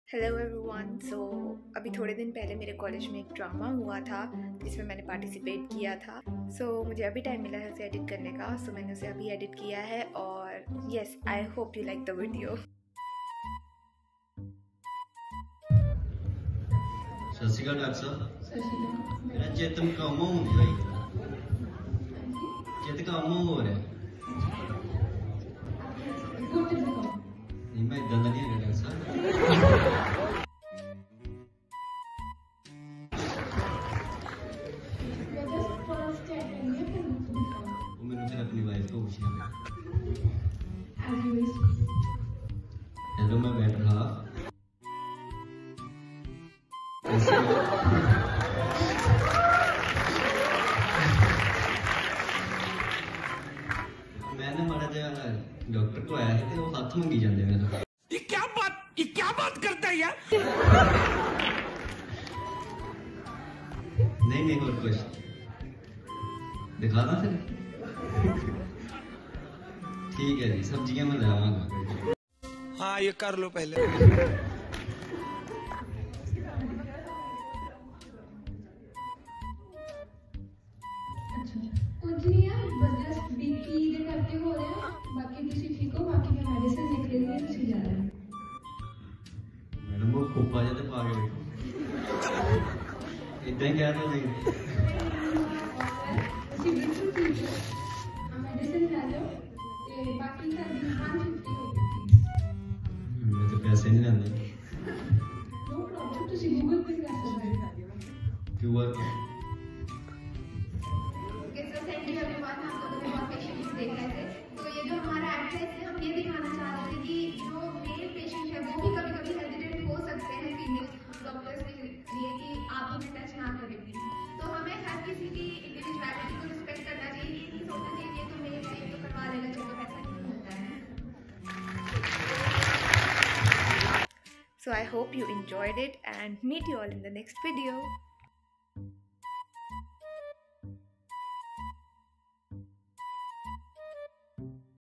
Skit/ Drama performed by MBBS students 2022 // GMC Amritsar // Government Medical College Punjab
Skit/ Drama performed by MBBS students 2022 during foundation course // GMC Amritsar // Government Medical College Punjab

Searches Related to:
Drama in medical college
Government Medical College
MBBS students 2022